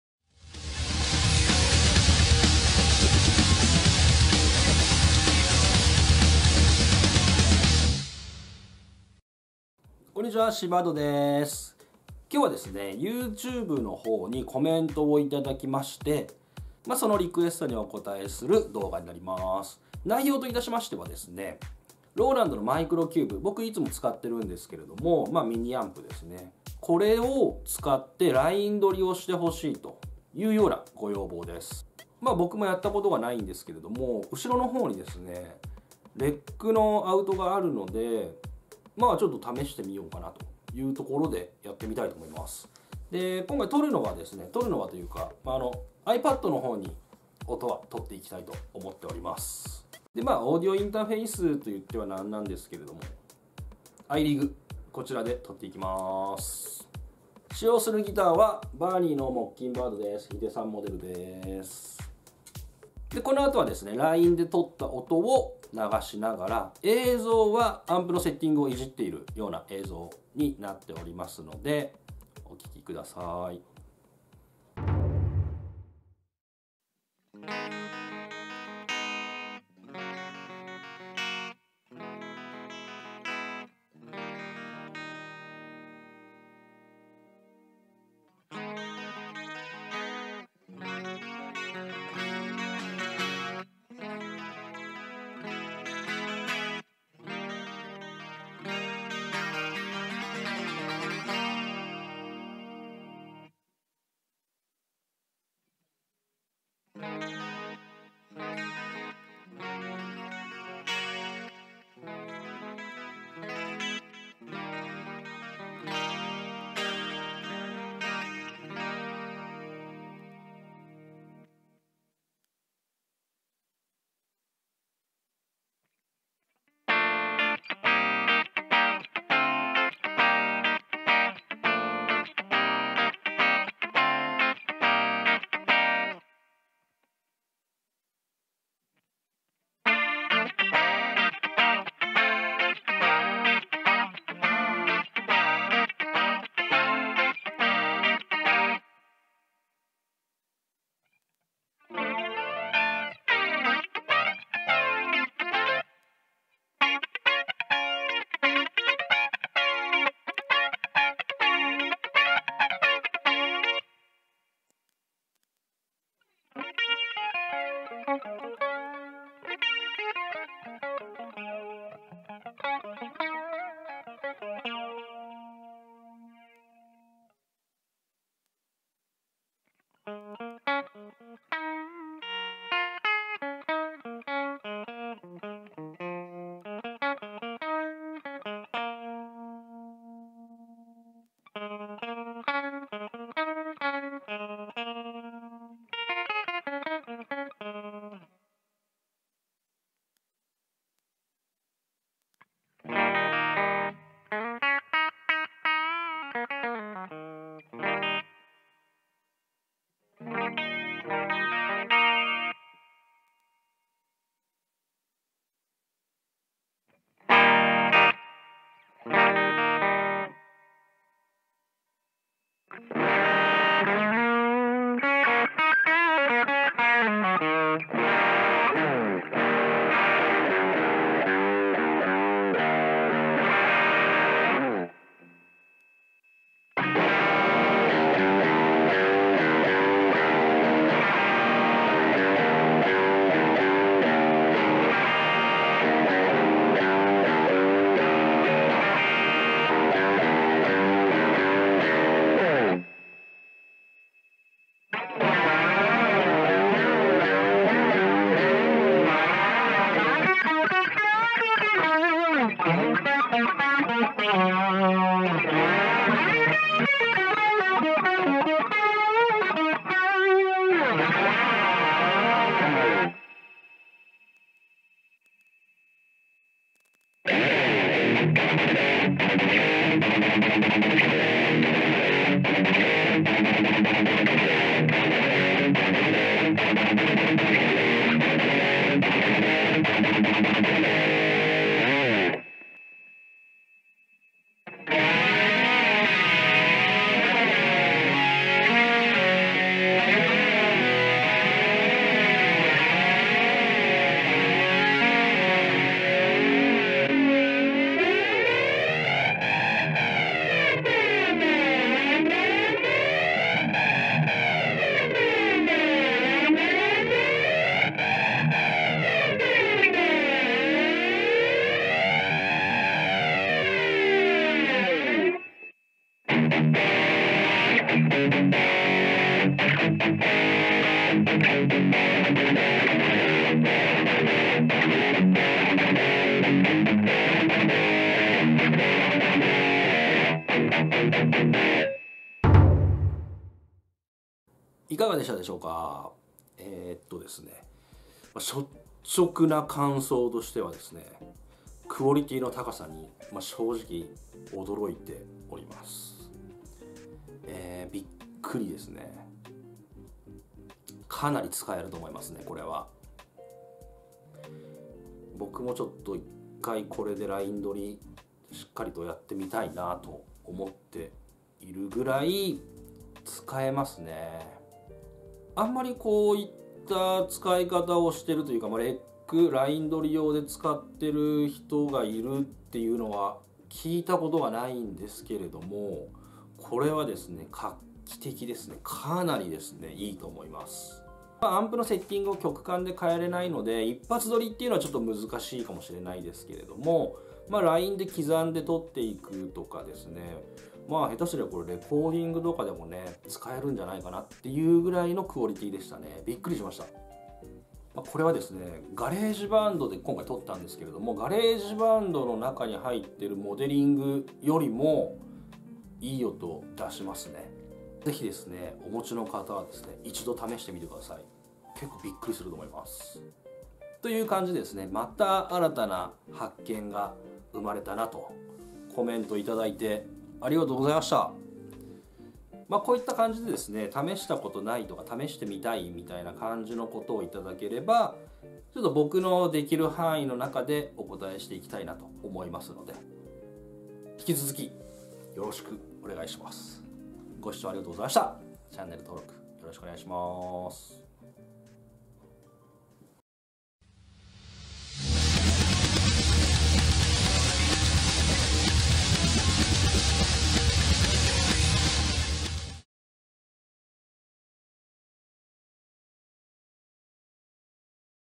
Roland MICRO CUBEでLINE録音してみたら【検証動画】Twitter質問箱リクエスト
Youtubeにコメント頂いたので、試してみました！

1977年生まれの私SeaBirdが、等身大の自分をお届けする新型音楽チャンネルです。